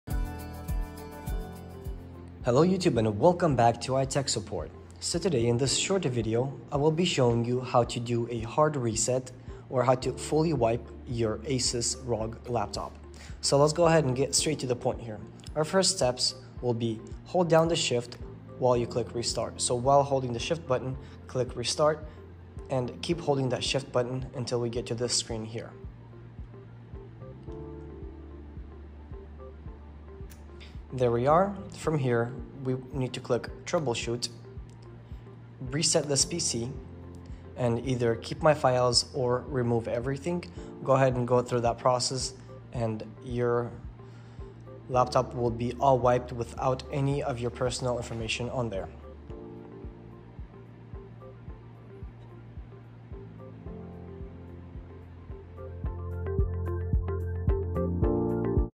How to Reset Asus Rog Laptop (Forgot Password? OK!
Short video shows you how to reset your Asus Rog Laptop